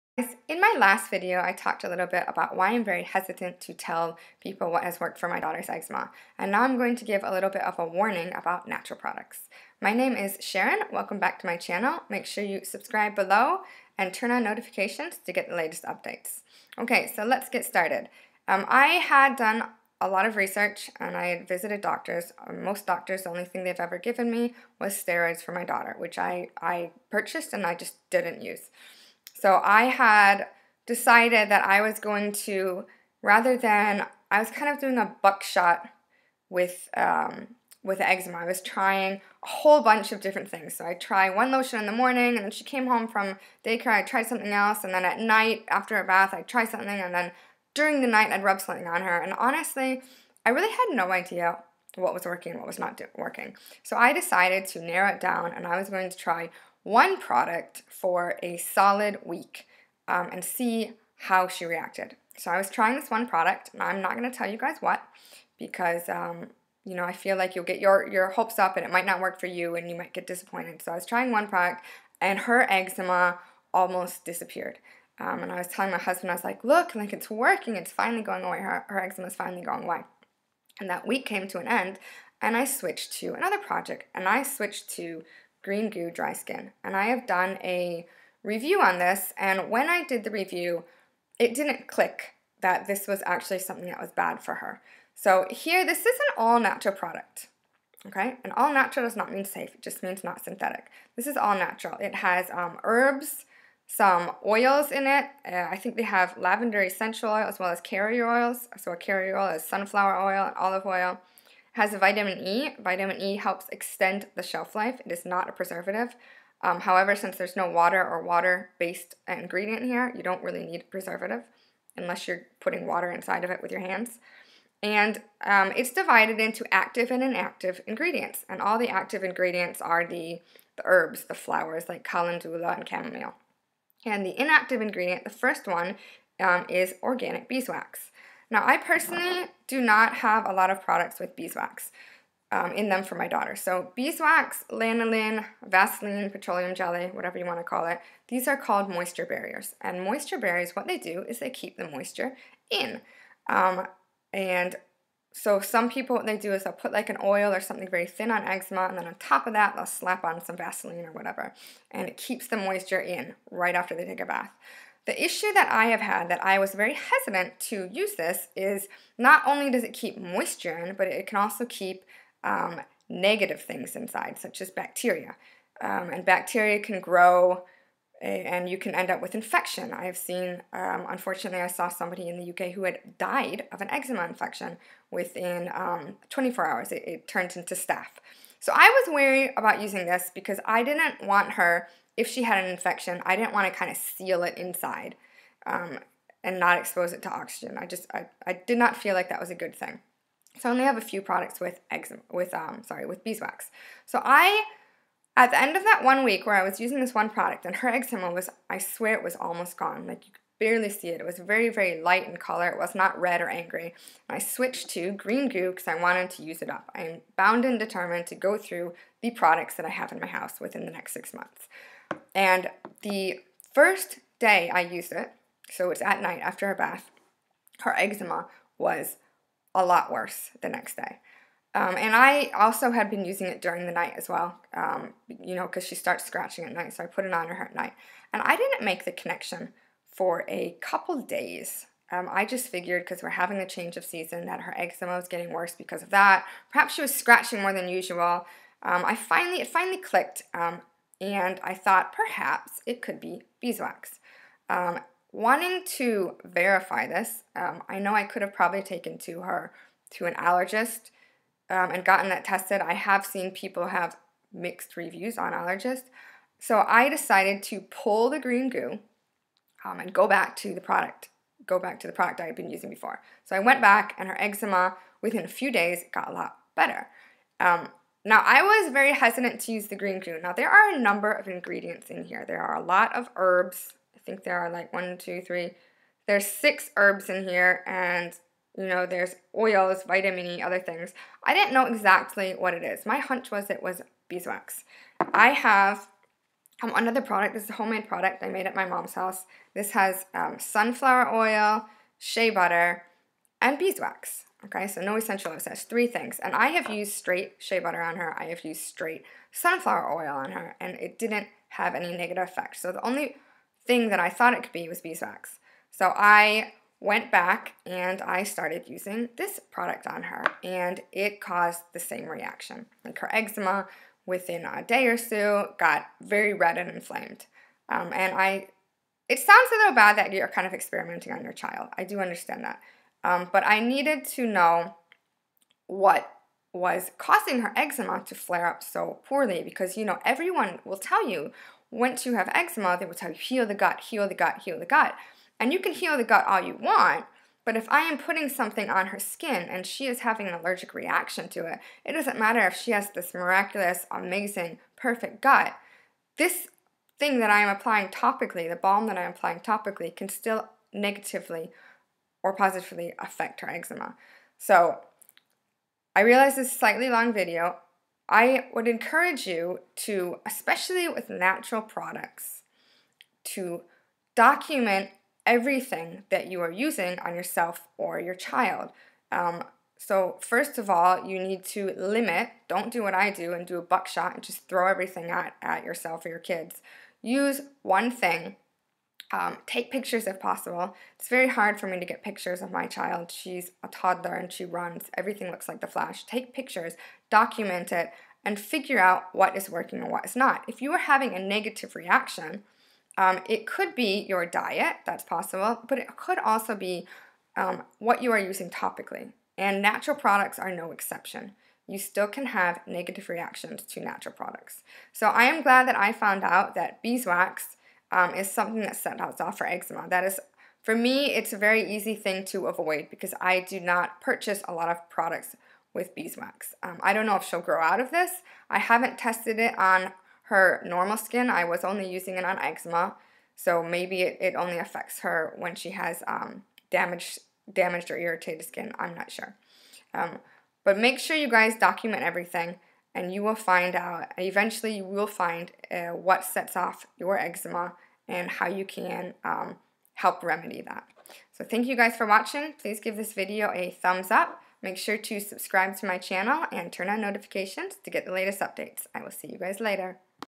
A WARNING ABOUT NATURAL PRODUCTS | BEESWAX IN ECZEMA REMEDIES
While beeswax may be natural. Natural does not always mean safe. Similar to lanolin or petroleum jelly, beeswax can act as a moisture barrier. I had been hesitant to use it since, while it may keep moisture in, it could also lock bacteria in. Thankfully, I do not have many products with beeswax in it due to this. I will not be using it on my child anymore since it causes her eczema to get worse.

My previous video talked about why I am hesitant to talk about what works for eczema, However, I feel it necessary to talk about the possible dangers of natural products. My daughter’s had a bad bad reaction to essential oils and you can see what happened

Eczema can be very severe and even cause staff infections, which could cause death.

Please seek the care of your medical provider if you believe your eczema infection is becoming severe.

SAFETY
Patch test products before use.

- MooGoo ($10 off your 1st order of $40+. Code MGRE-SHAR-DB2A27)